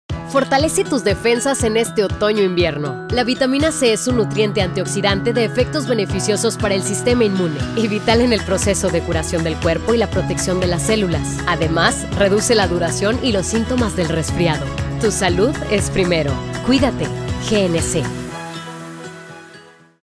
Fortalece tus defensas en este otoño/ invierno
La vitamina C es un nutriente antioxidante de efectos beneficiosos para el sistema inmune y vital en el proceso de curación del cuerpo y la protección de las células, además reduce la duración y los síntomas del resfriado.

Tu salud es primero ¡Cuídate! GNC